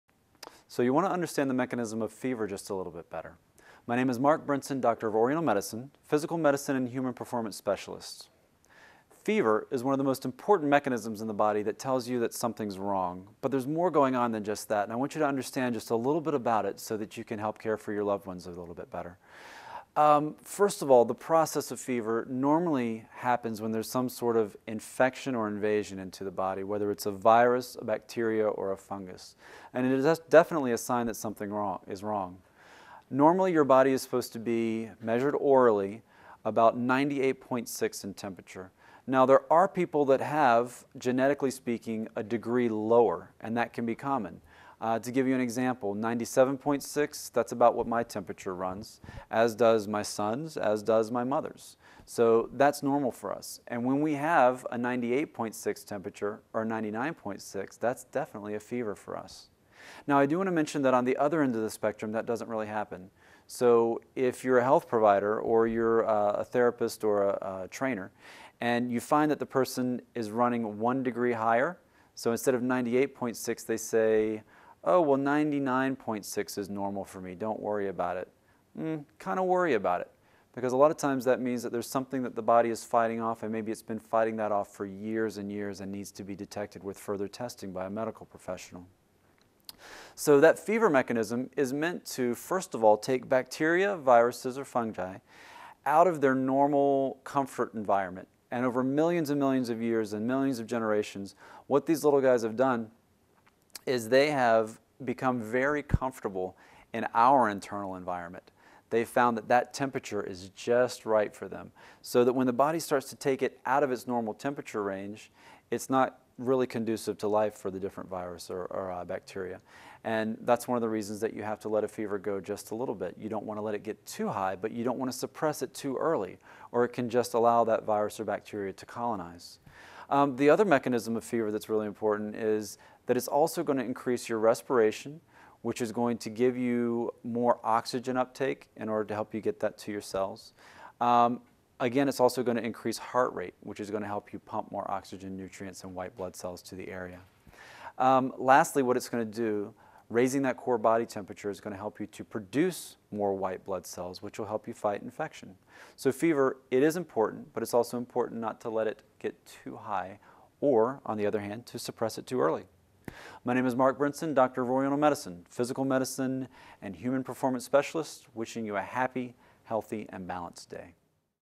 Natural Remedies & Cleanses : How to Understand a Fever
A fever typically tells the body that something internal is going wrong, and the rising temperature helps to fight off unwanted bacteria or infection. Understand how a fever helps fight off infection with tips from a doctor of Oriental medicine in this free video on natural remedies.

Bio: Mark Brinson owns Brinson Therapeutics. He specializes in pain, injury and human performance and is also an international seminar provider.